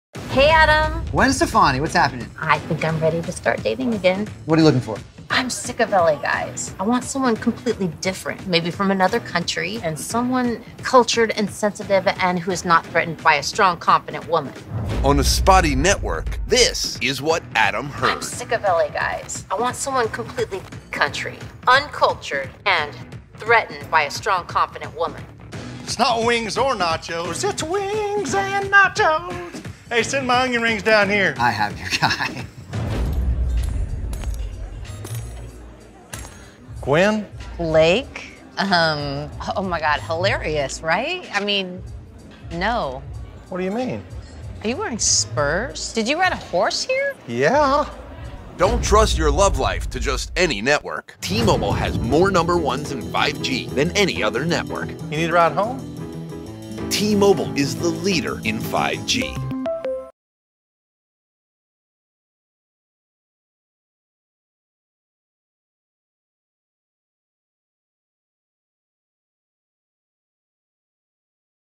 T-Mobile Super Bowl Commercial 2021 Gwen Stefani, Blake Shelton, Adam Levine
T-Mobile Super Bowl Commercial 2021 Gwen Stefani, Blake Shelton, Adam Levine. You can watch T-Mobile Super Bowl 2021 Commercial featuring  Gwen Stefani, Blake Shelton and Adam Levine. After a video call on a spotty network, Adam Levine takes what he heard and sets up Gwen Stefani and Blake Shelton on what he thinks will be the perfect match.

T-Mobile Super Bowl Commercial 2021